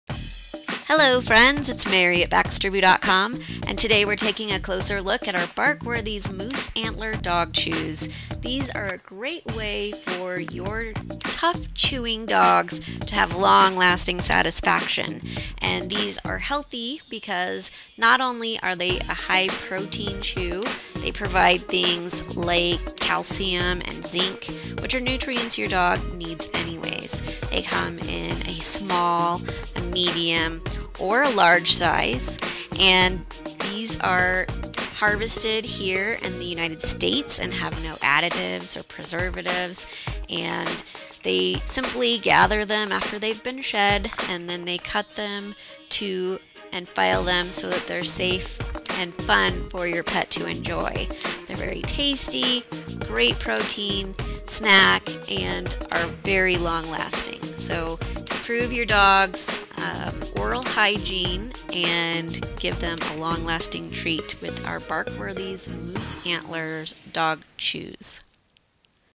Barkworthies Moose Antler Dog Chew
Give your pup a healthy natural chew that's long lasting and great for their teeth with our Barkworthies Moose Antler treat.